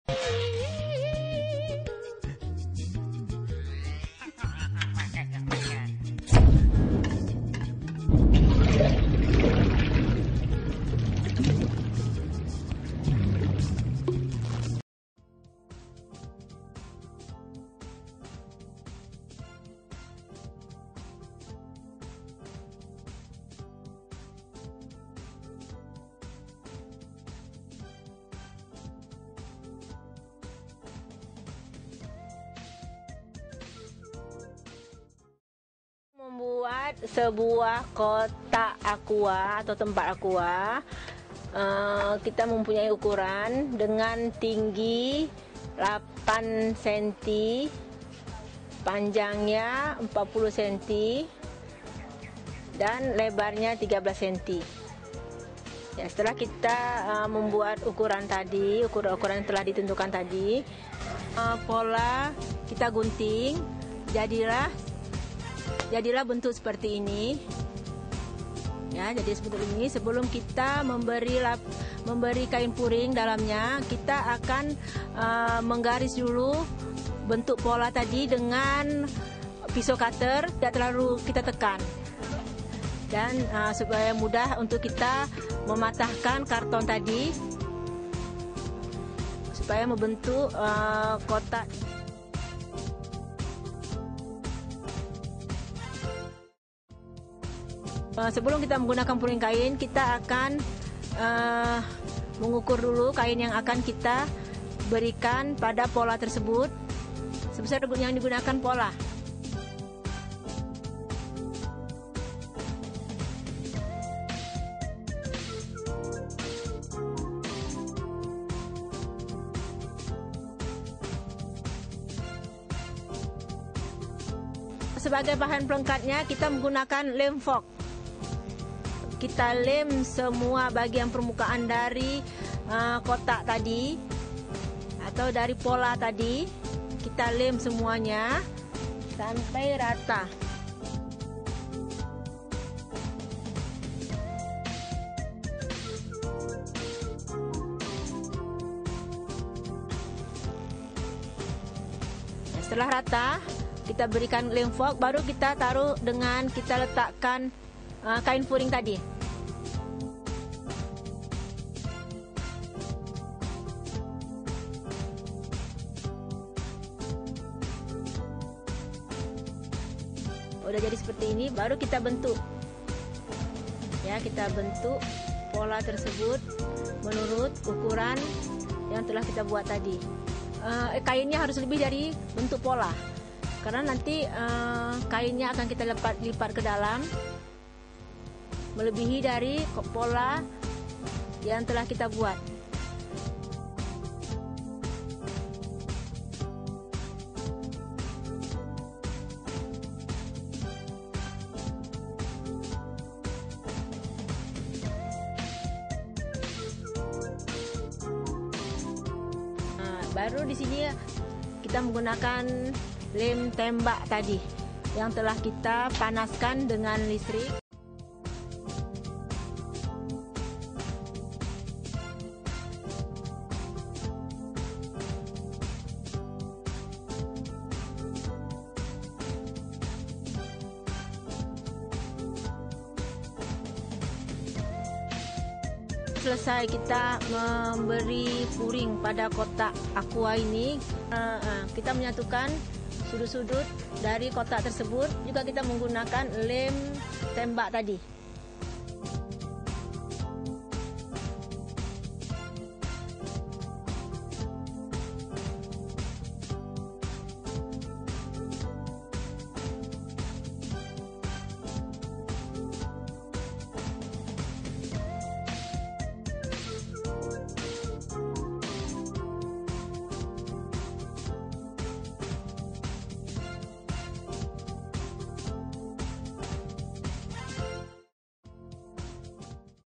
G1101 Step 3 Covering the Box ID
A tutorial video about how to make decorative boxes